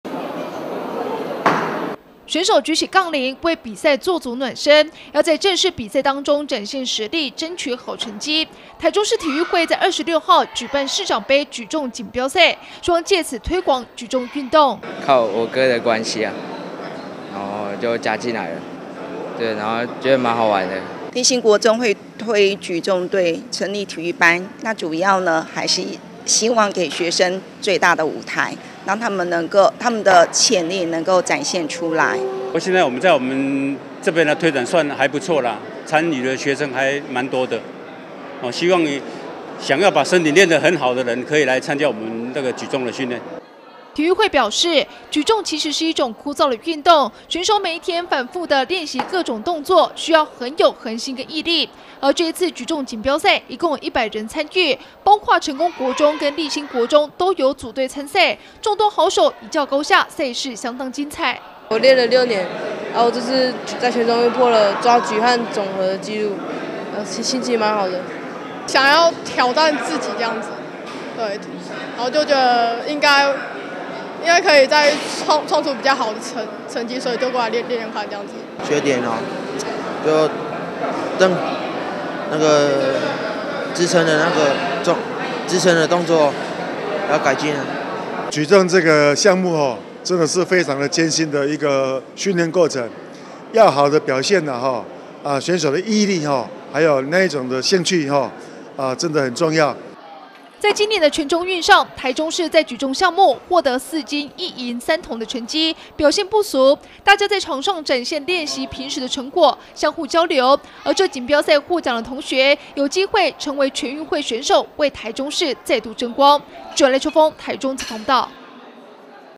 大台中新聞-大里立新市長盃舉重錦標賽
為了進一步推廣舉重運動，台中市體育會，26號在大里立新國中，舉辦一場錦標賽，包括成功以及立新國中等學校，加起來約1百人參加，大家在場上相互較勁，展現平時練習成果，也藉此交流，讓實力再度提升。「以上新聞可從哈TV APP收視」。